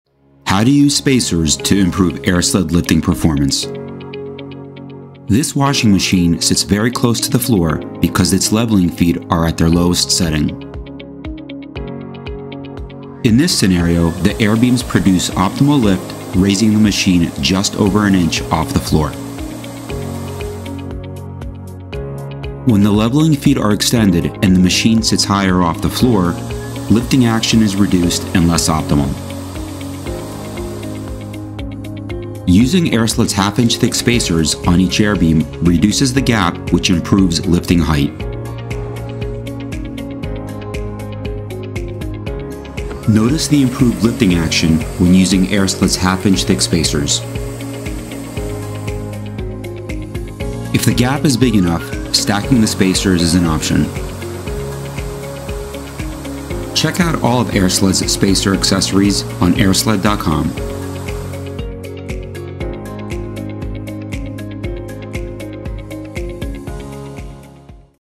How to Achieve Optimal Airsled Lifting Performance
This video illustrates how spacers can improve lifting height and overall Airsled performance when moving items that are more than 1/2" off the floor.